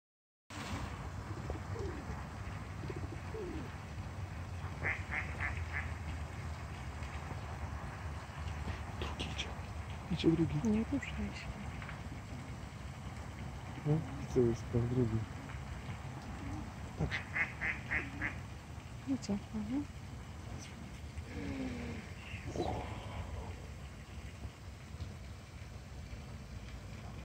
Szczury w parku. Oława
Mieszkańcy zauważyli, że z parku przy PKP jest coraz więcej gryzoni. Zgłaszają sprawę redakcji, ponieważ obawiają się o dzieci, które korzystają z pobliskiego placu zabaw